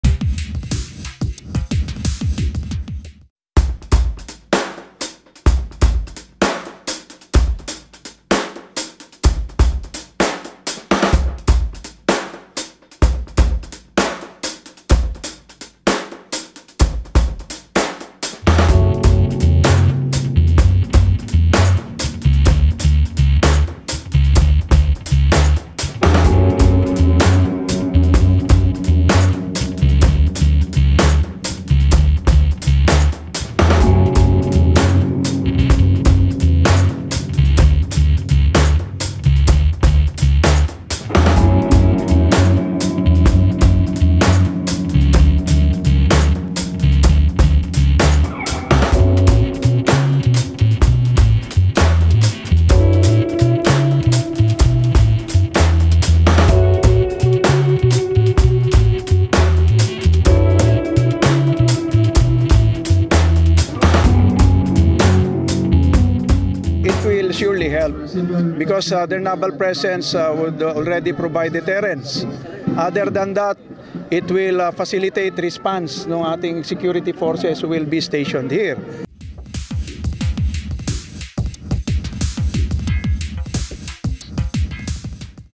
Philippine Navy's newest Fleet-Marine Detachment to rise in Zamboanga City
The Philippine Marines is establishing its first detachment in Zamboanga City that will be responsible to monitor the activities of lawless elements in the region.

The Department of National Defense (DND) has held a groundbreaking ceremony in Zamboanga for the first fleet detachment of the Philippine Marines in barangay Rio Hondo...